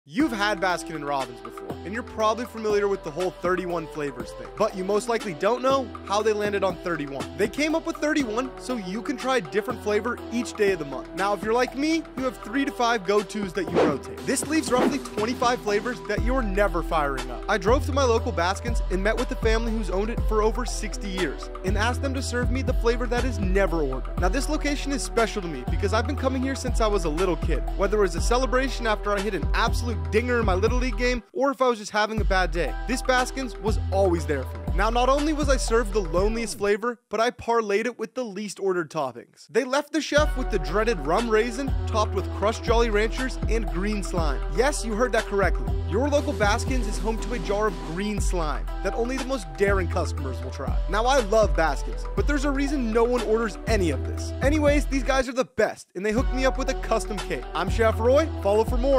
Why does nobody try this ice cream
Going to my local Baskin-Robbins and ordering the least ordered ice cream and toppings.